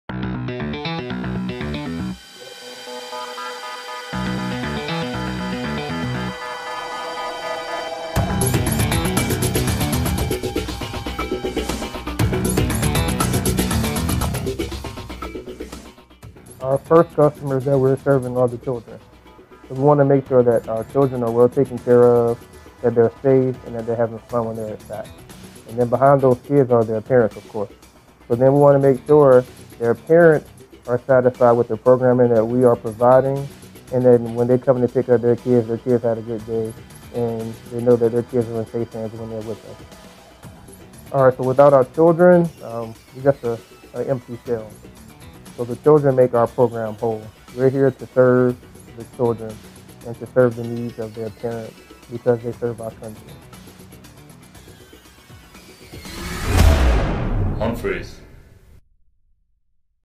NYK – Service Culture (DeAngelo)
Credit: Jay Mann | Date Taken: 04/06/2020
Now You Know - DeAngelo from SAC responds to a positive ICE Comment he received. He says, “Without the children we are just an empty shell...the children make our program whole.